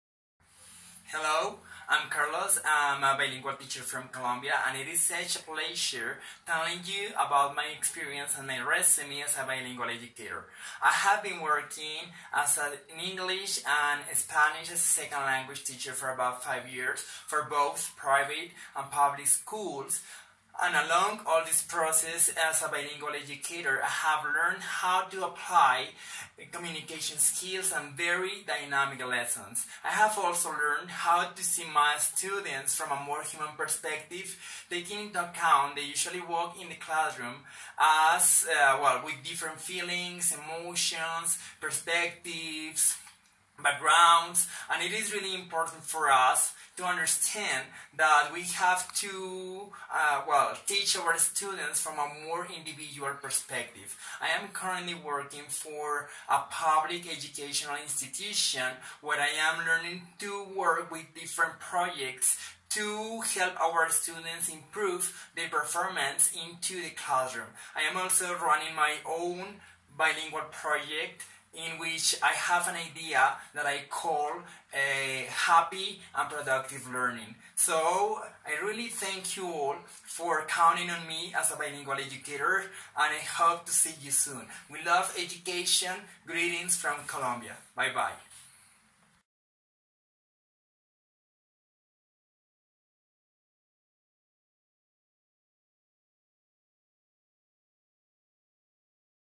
Brief introductory video - learning English and Spanish
English and Spanish as a Second Language Teacher with more than seven years of experience teaching language skills to students from all ages. Having been immersed into the educational language field all these years have taught me the real meaning of teaching and learning a language through communicative skills and making the lessons as dynamic as possible for students to enjoy learning while being empowered to discover their abilities into the language.